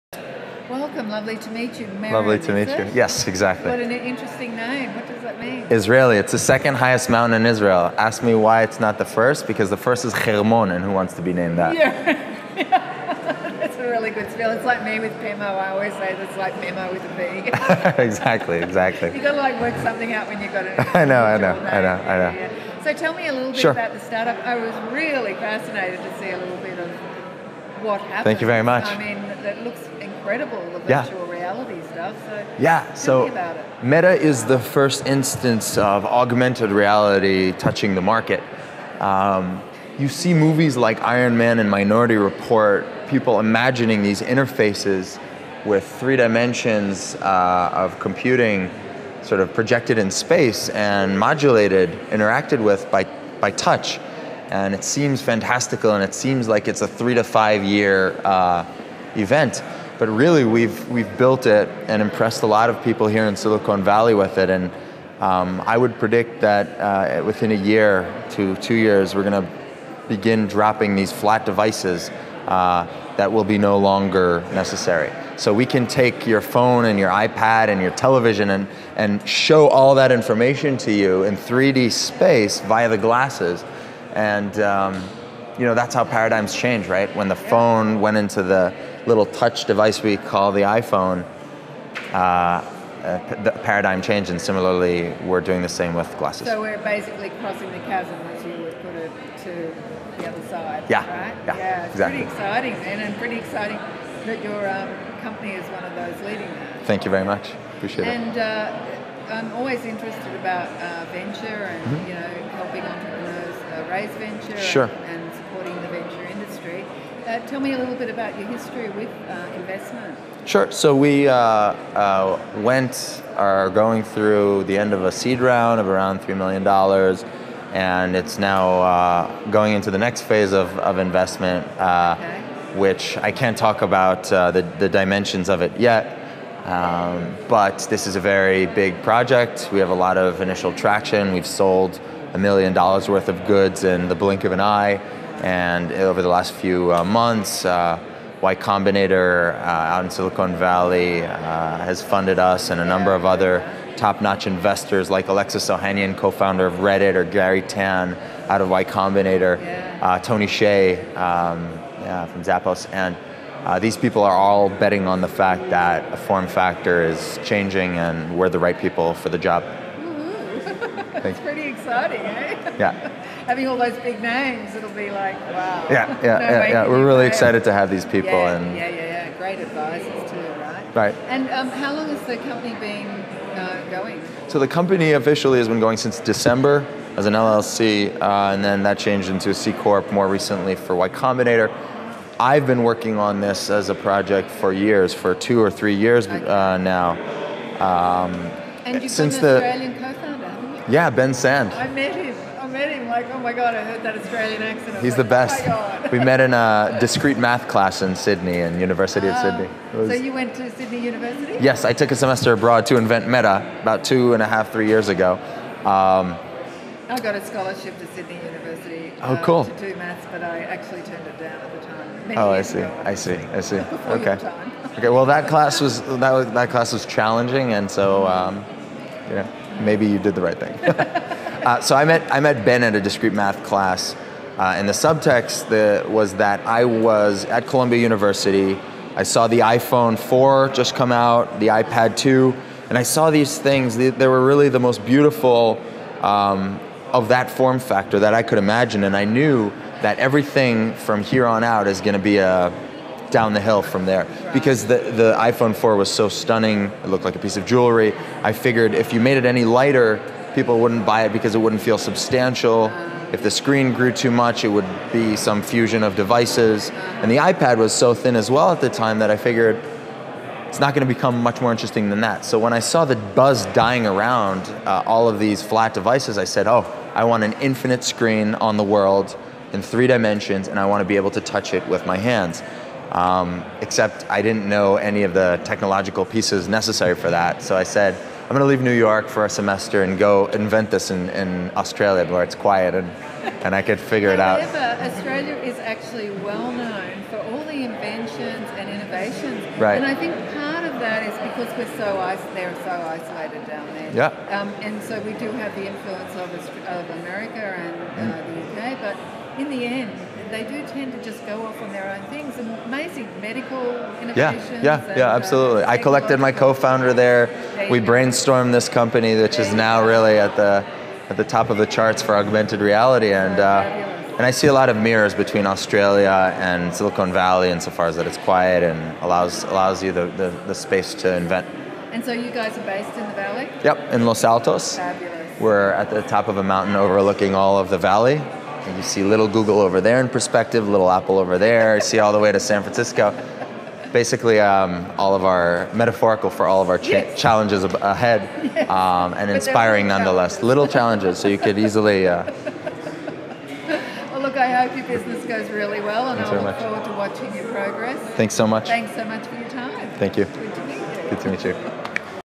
Meron Gribetz, CEO/Founder meta
I interviewed Meron Gribetz, CEO/Founder meta @GLAZED Conference.  Meta can show phone, laptop & other device info & show in 3D space via the glasses. The paradigm is changing.  He spoke about how they have raised their Seed Round & are not about to do Series A. They have sold $1m worth of goods over the last few months.  YCombinator have funded them.  He has an Aussie CoFounder whom he met when he was studying maths @Sydney University.  He invented meta in Oz.